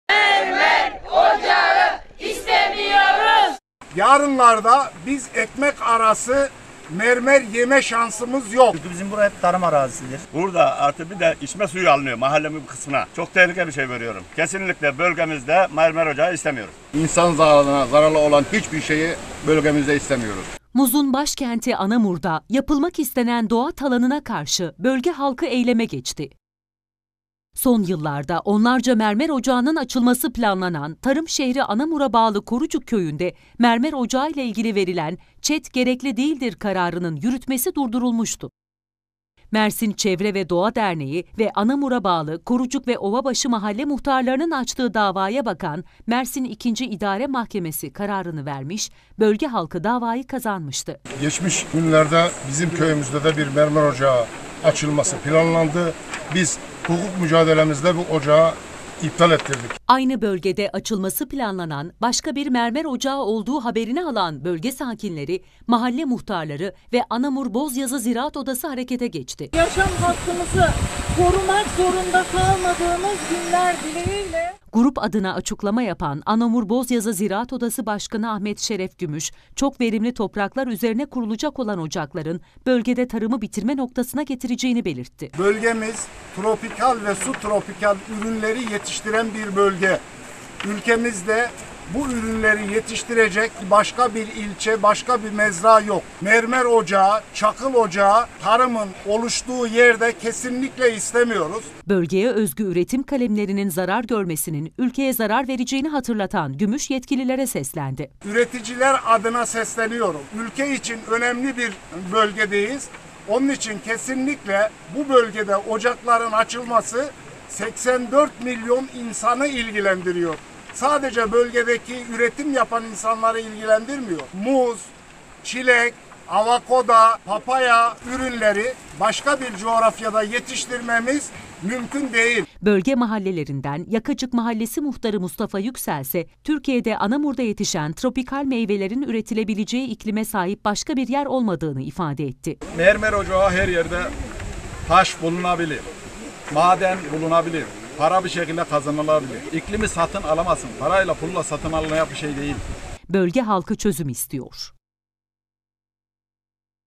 Mersin Anamur'da mermer ocağına karşı köylüler eylemde
Mersin’in Anamur ilçesinde bölgenin tarihi dokusuna ve doğasına zarar verecek olan mermer ocaklarına, tepki büyüyor. Mermer ocağının yapılacağı alanda eylem yapan köylüler Anamur bölgesinde tarım üretiminin sağlıklı devam edebilmesi için ocakların kurulmasını istemiyoruz” dedi.